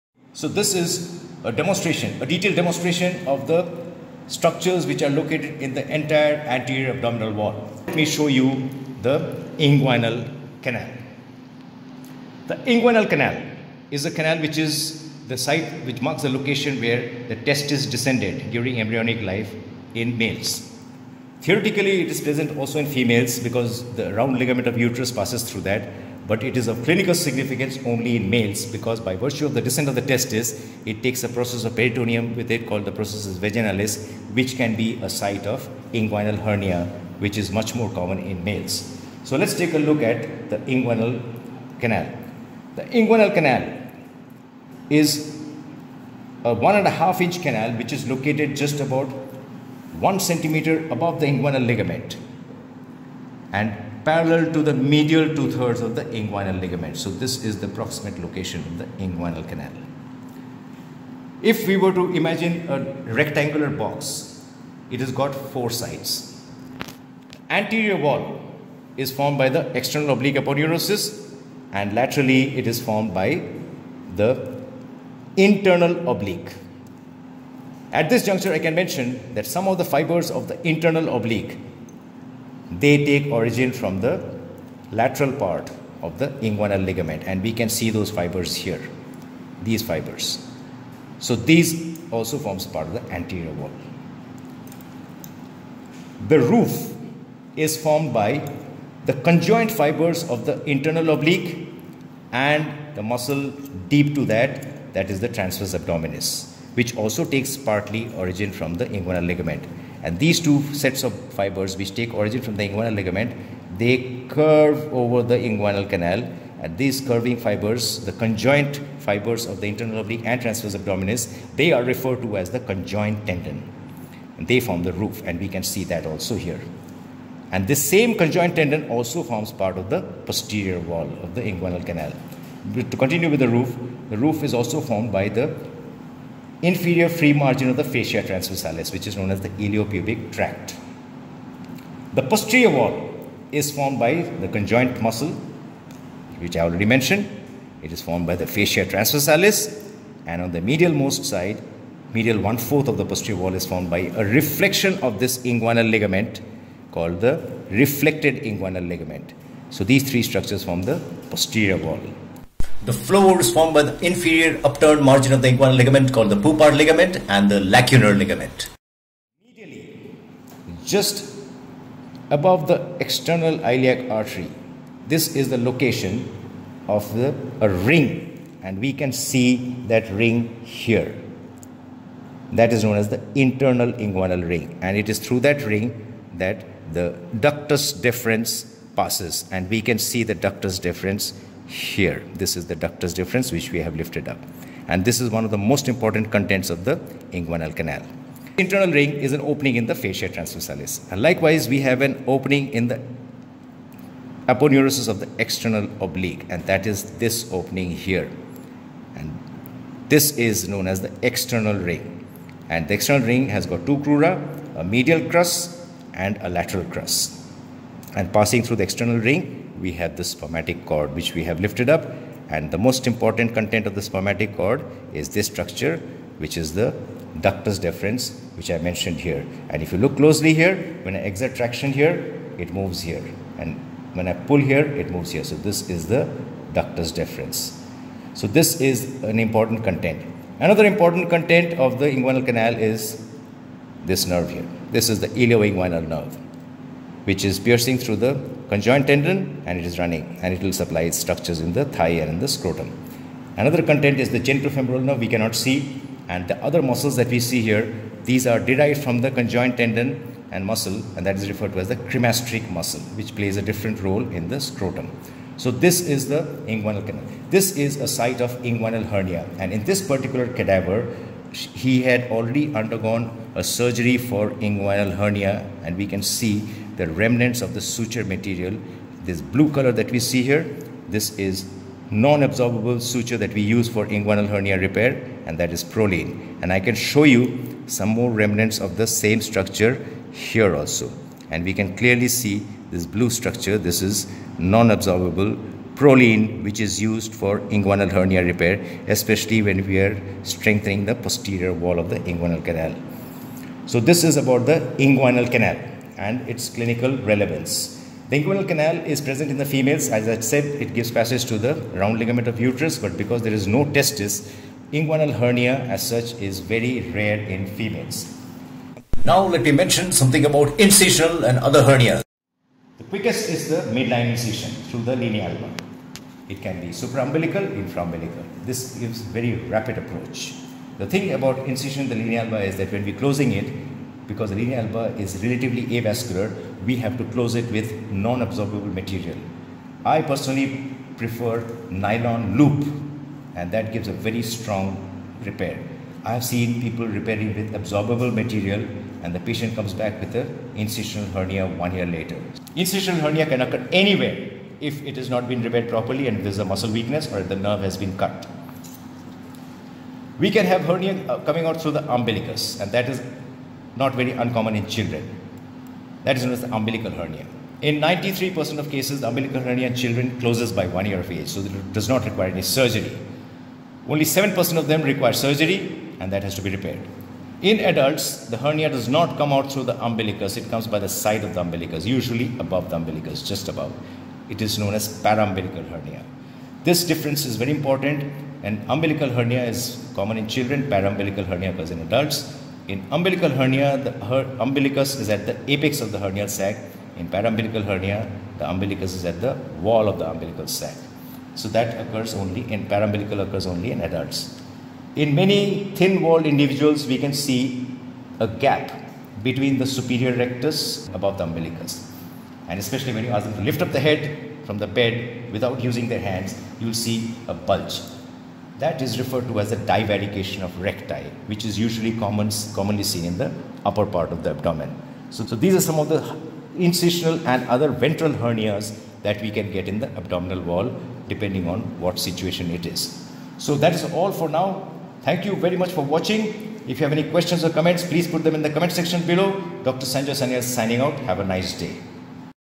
INGUINAL CANAL and Abdominal Ventral HERNIAS – Sanjoy Sanyal
This video shows the detailed architecture of the Inguinal Canal. The specimen was harvested from a cadaver.
Concepts of Inguinal and other ventral abdominal hernias are also described
Cameraperson was an MD 3 student using my Samsung Galaxy S8 with its 12 MP rear and 8 MP front-facing cameras. Your acquiescence in this regard is much appreciated!